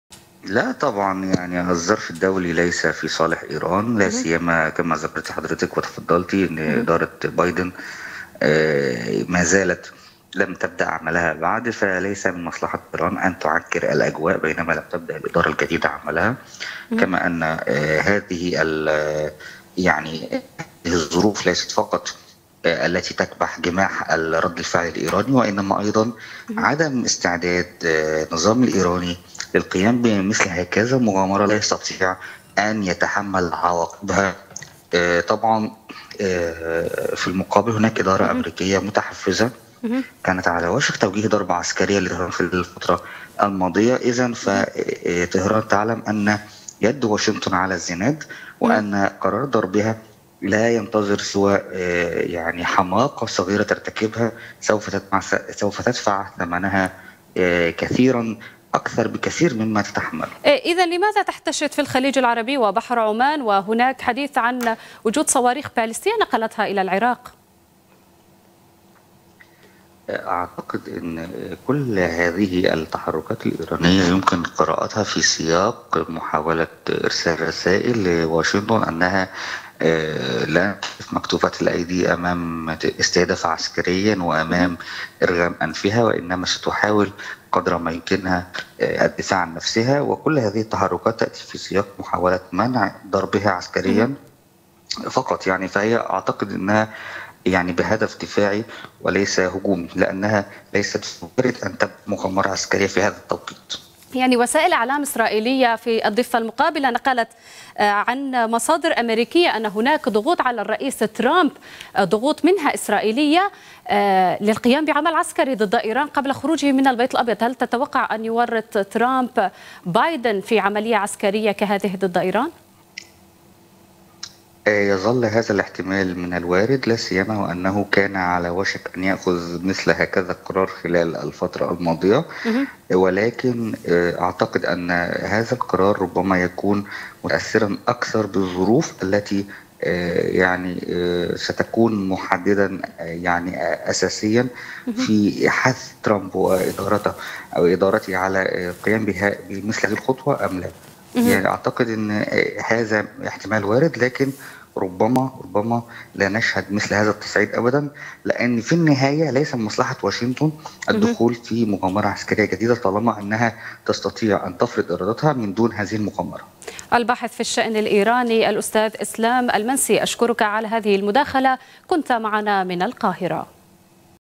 إسلام المنسي:التحركات الإيرانية بمنطقة الخليج هي رسائل للولايات المتحدة أنه بإمكانها الدفاع عن نفسها
القاهرة
إسلام المنسي ـ باحث في الشأن الإيراني: التحركات الإيرانية بمنطقة الخليج هي رسائل للولايات المتحدة أنه بإمكانها الدفاع عن نفسها في حال استهدافها عسكرياً، لكن ليس الغاية منها شن هجمات.
ــــــــــــــــــــــــــــــــــــــــــــــ

تابعوا قناة اليوم على التردد:
 12687_27500 (H) _NILESAT